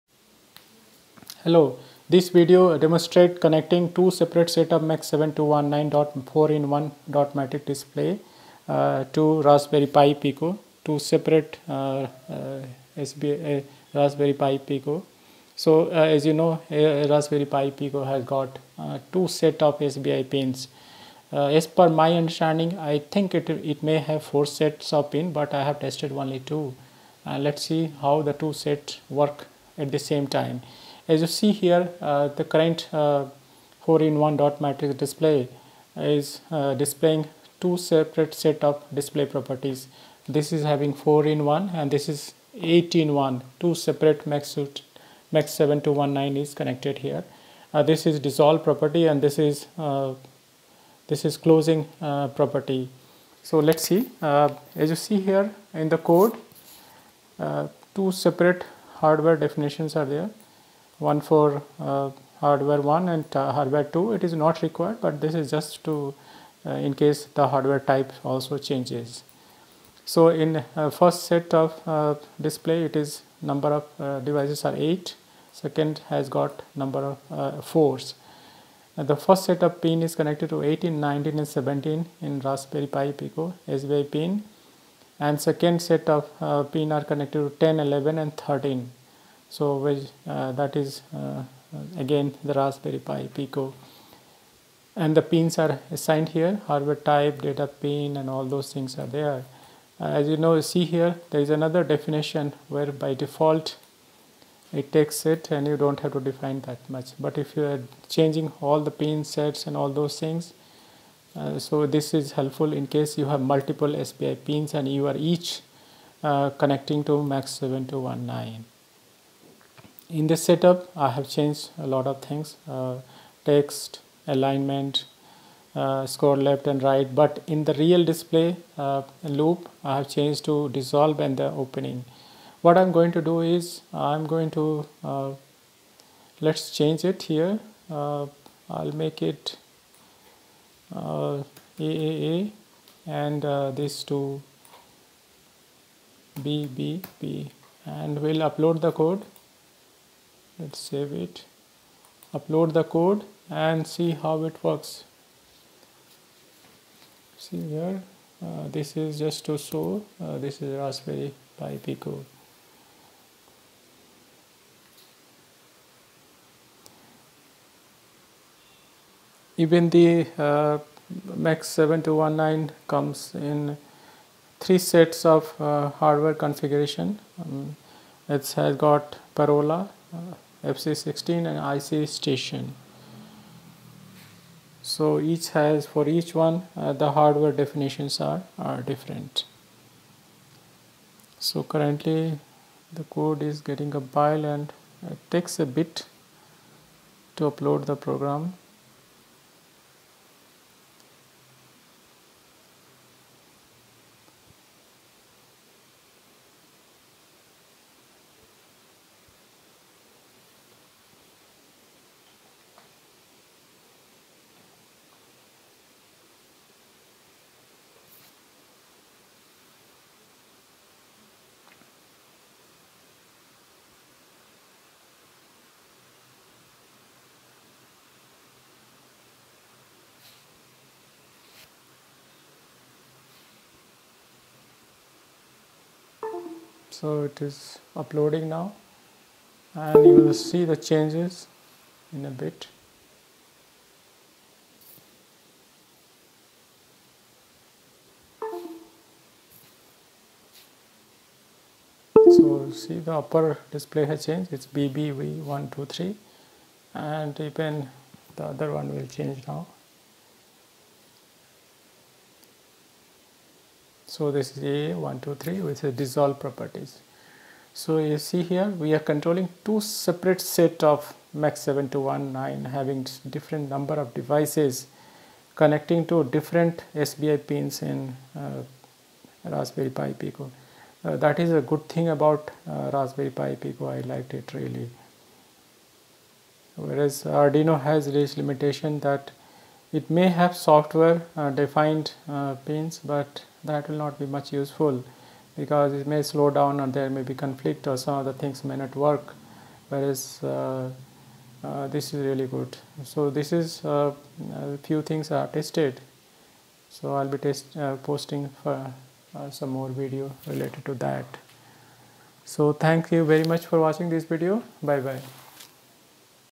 How to connect multiple MAX7219 4-IN-1 DIGITAL display to Raspberry Pi Pico ?
This is very unique video in that it demonstrates displaying two MAX7219 4 in 1 digital display using Raspberry Pi Pico. The no of MAX7219  devices are different  and they are using the same SPI interface with Rasp berry PI Pico.
The hardware for the MAX7219 is same.

Accessories needed :
1.Many MAX7219 4 in 1 display atleast 3 sets
2.Raspberry PI Pico
3.Wires
4.misc